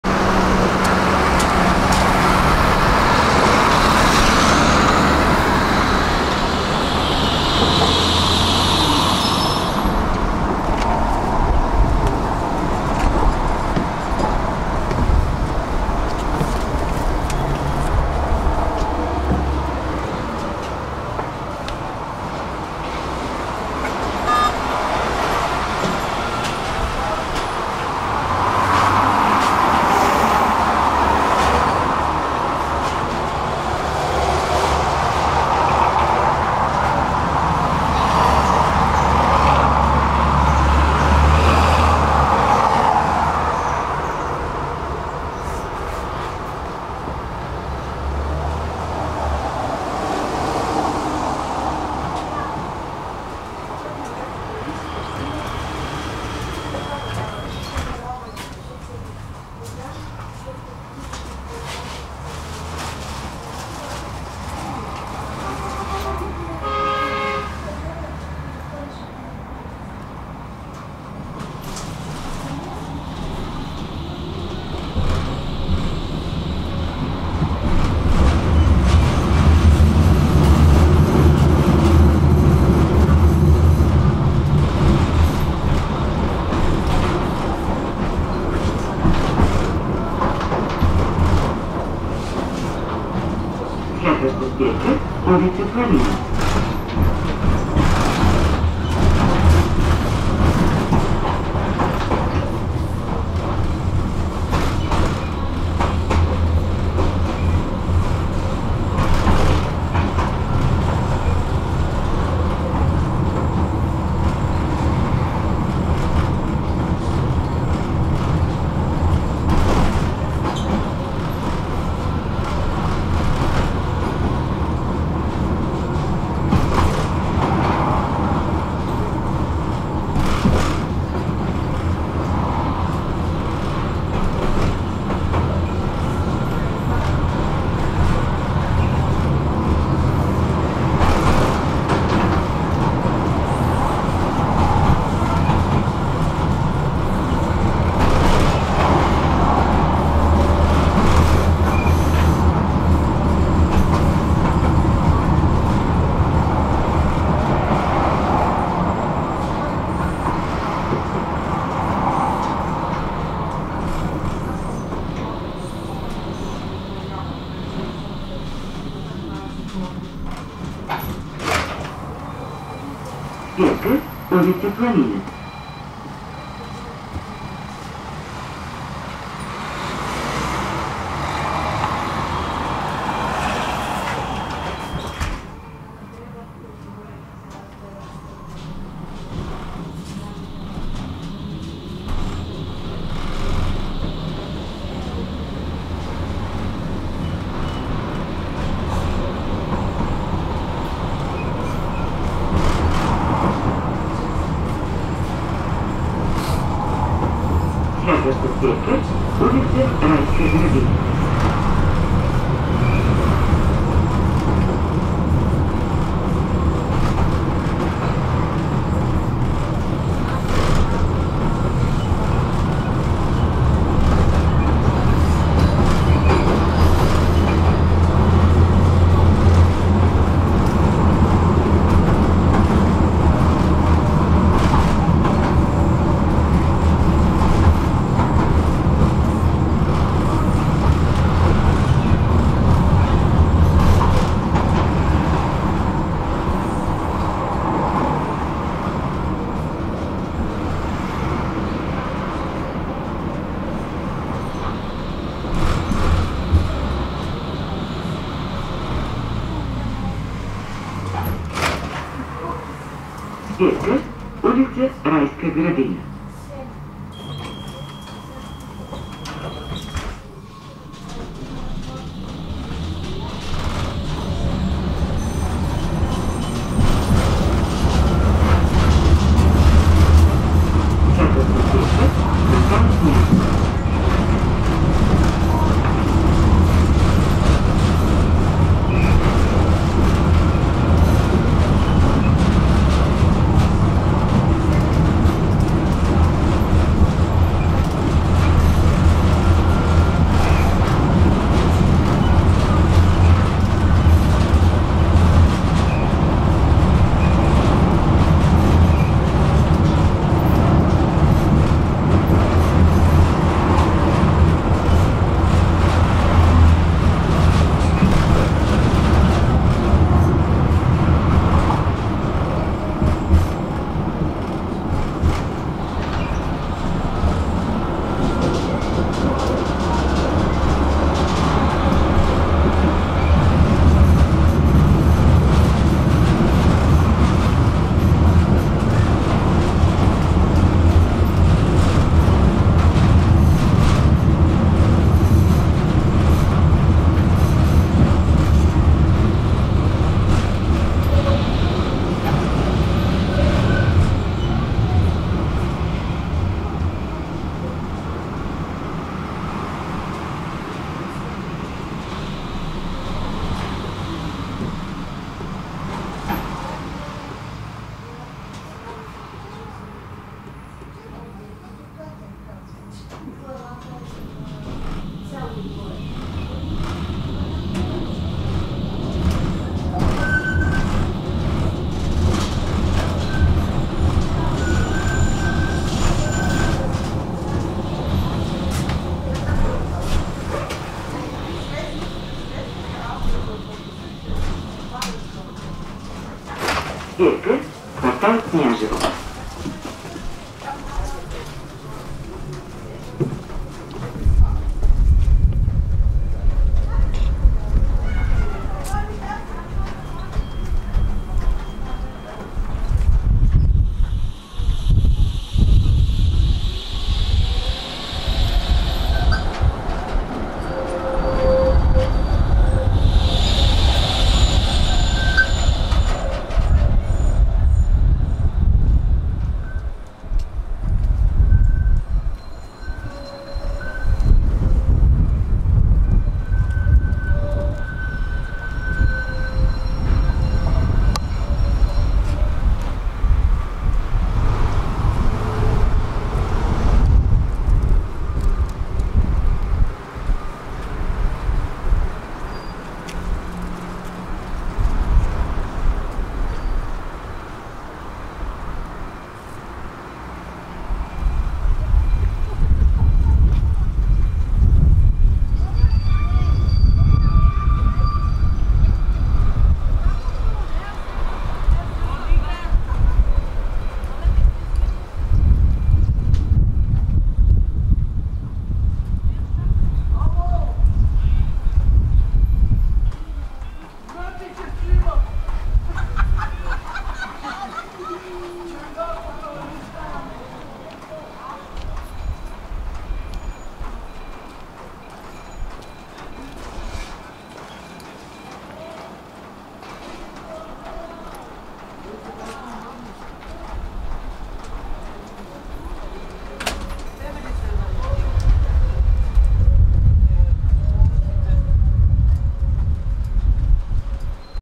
Bulgaria, Sofia, tram ride 5 from bul. Nikola Petkov to kv. Knyazhevo
March 2019